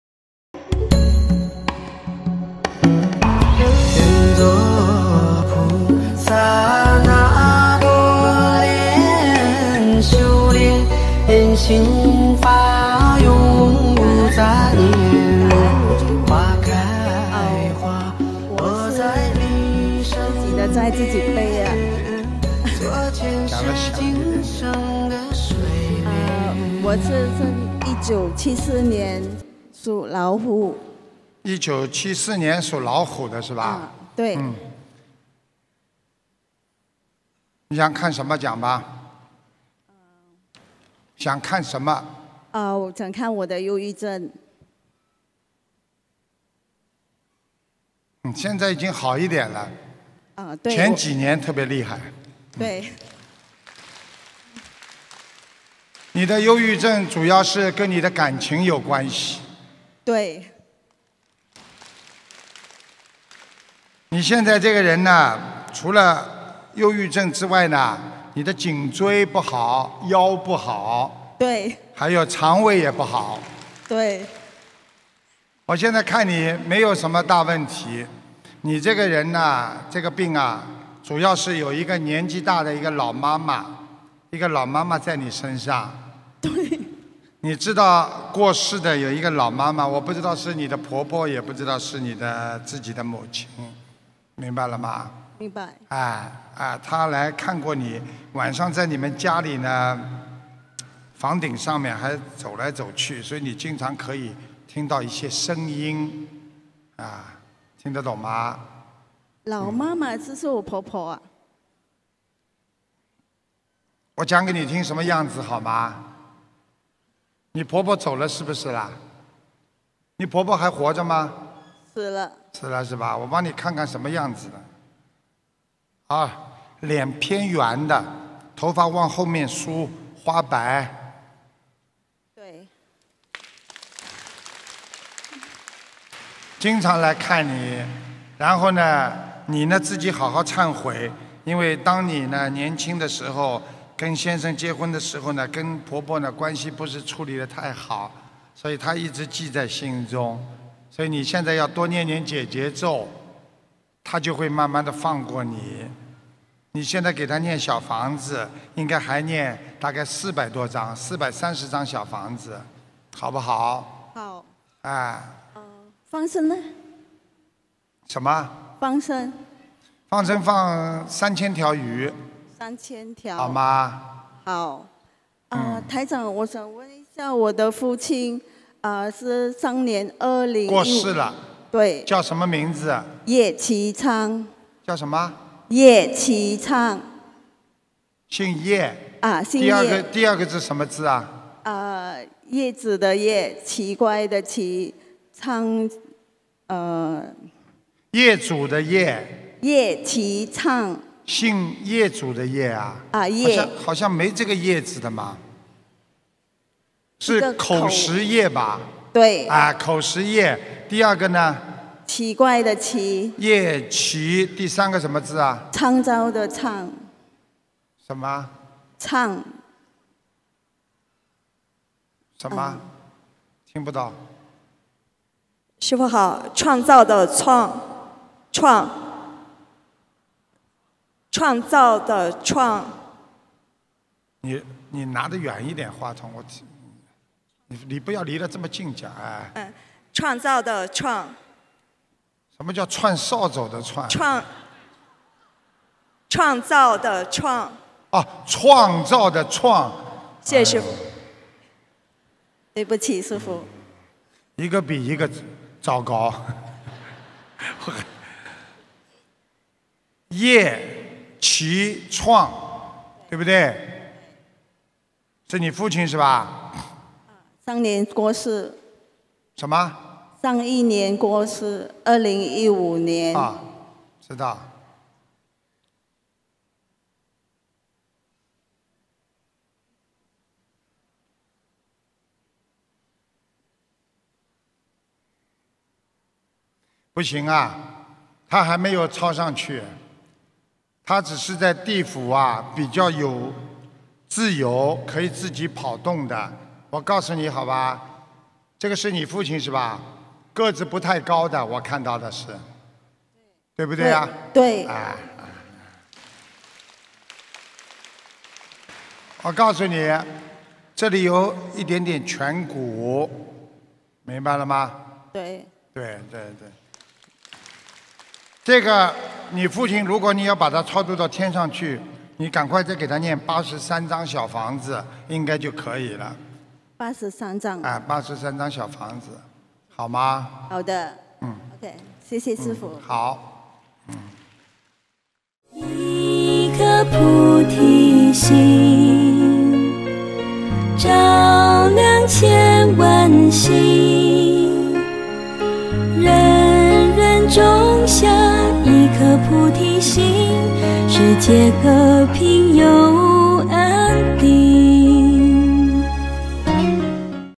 【20160820馬來西亞馬六甲看圖騰精選】婆媳關係不好；感情問題導致憂鬱症。
婆媳關係不好，婆婆亡故後不放過媳婦；
感情問題導致憂鬱症。